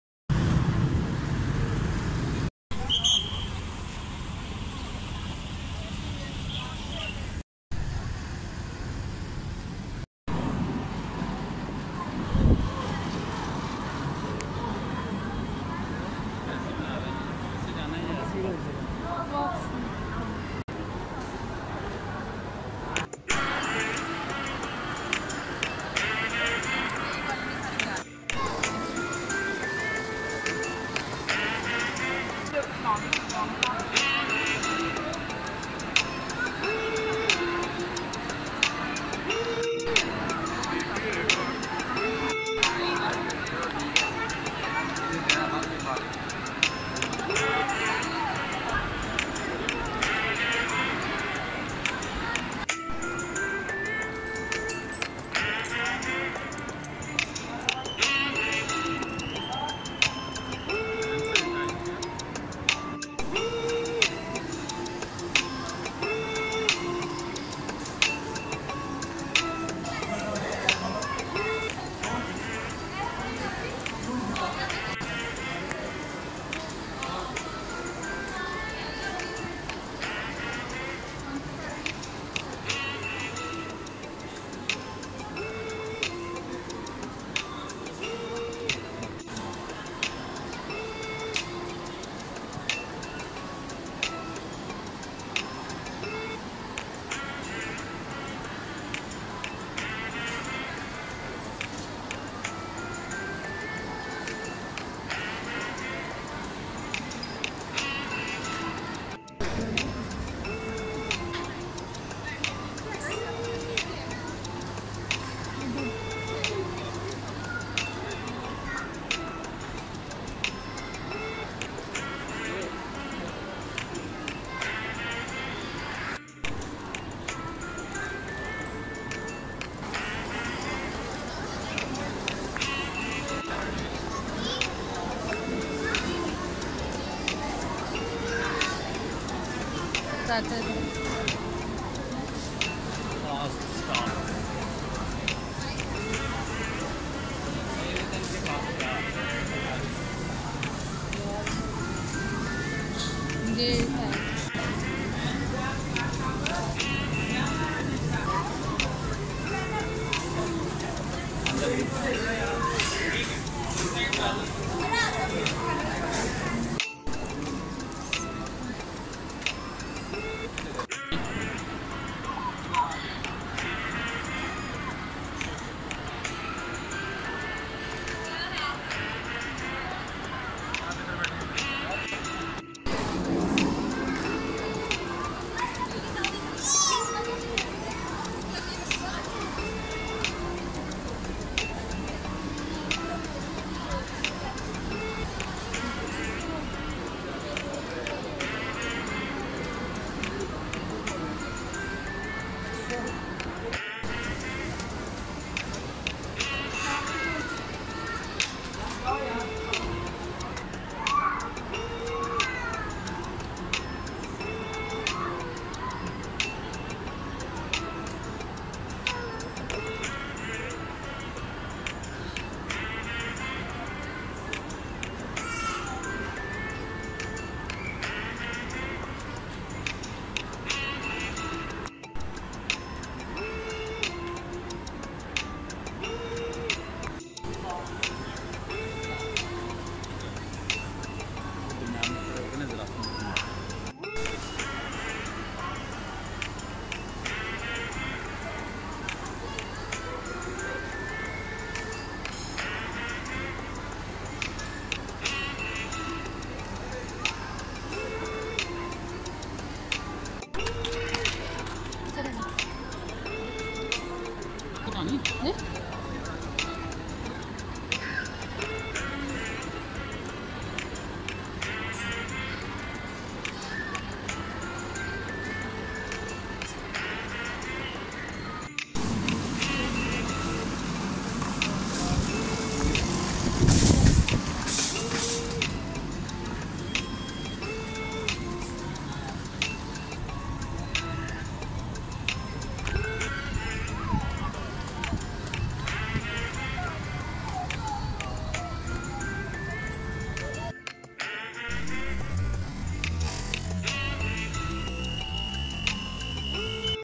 Biggest Shopping Mall of Lahore || Emporium Mall Lahore - Largest Shopping Mall by Nishat Group.
Emporium Mall Lahore - Largest Shopping Mall in Lahore Pakistan By Nishat Group. 200+ Brands. Shops Restaurants and playing areas